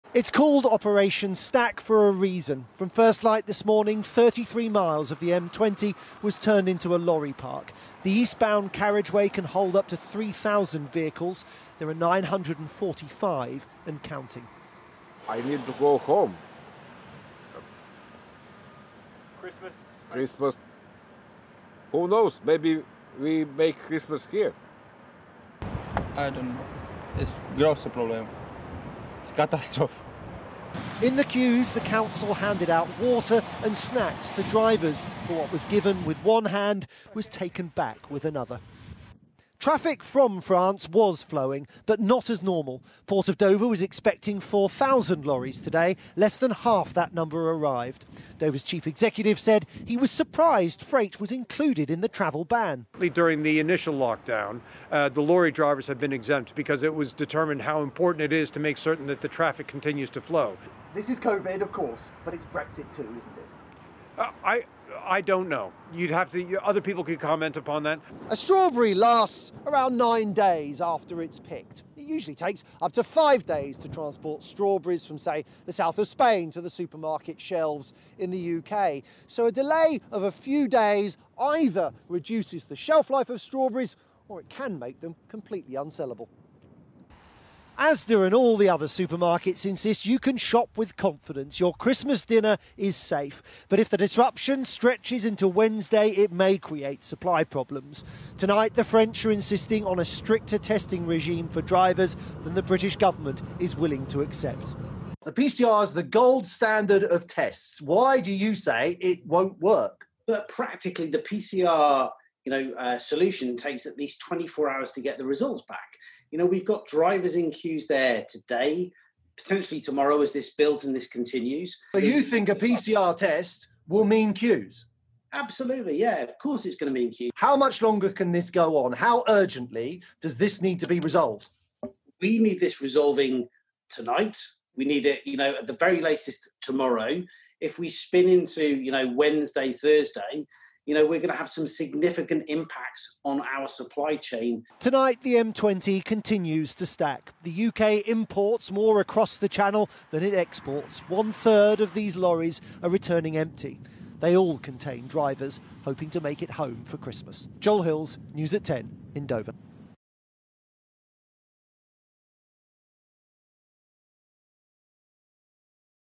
Covid: No sign yet of solution to lorry lockdown at UK border | ITV News
As the prime minister said, he and the French President, Emmanuel Macron, are keen to get the lorries rolling again, in the next few hours, if possible, although there is no sign of that  yet.

Mr Johnson says 80% of the freight arrives in containers loaded onto ships, so without drivers. It is fresh food that arrives in lorries.

The Supermarkets say they are all right for now and Christmas lunch is not at risk.

But Sainbury's and Tesco said lettuces, cauliflowers, broccoli and citrus fruit may run short in time.